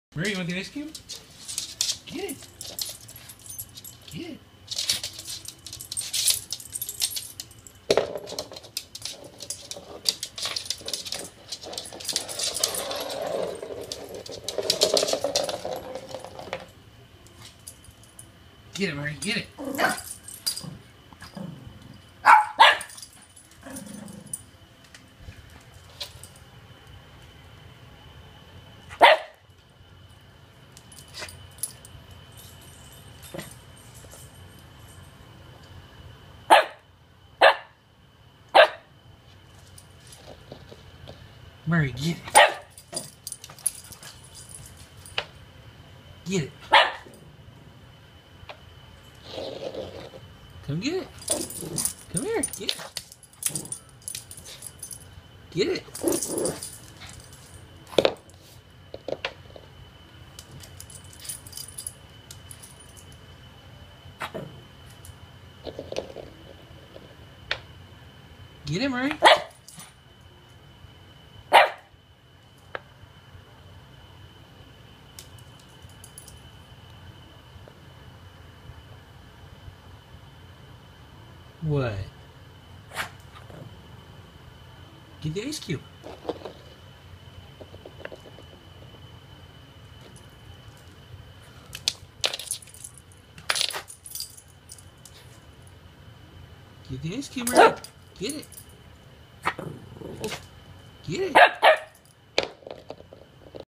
Murray and the Ice Ball
Our dog Murray loves to lick ice cubes. To mess around with him, today I gave him a cocktail ice ball instead. I don't think he appreciated it.